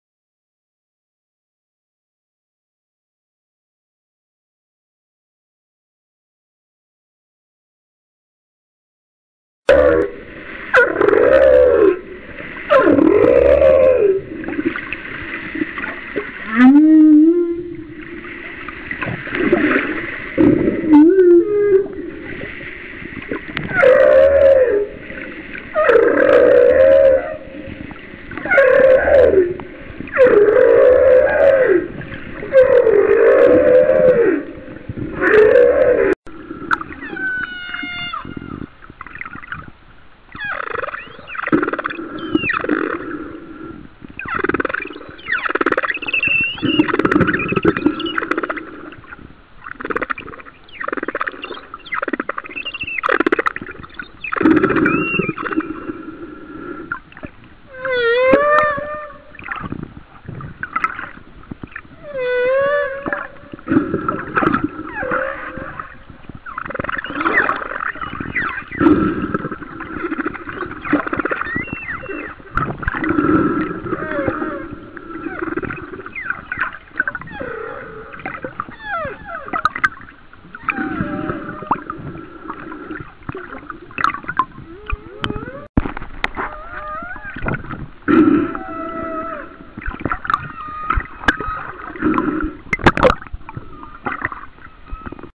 Humpback Whale Songs - Kauai 2009
We actually filmed this ourselves about 1 mile off of the  coast of Kauai.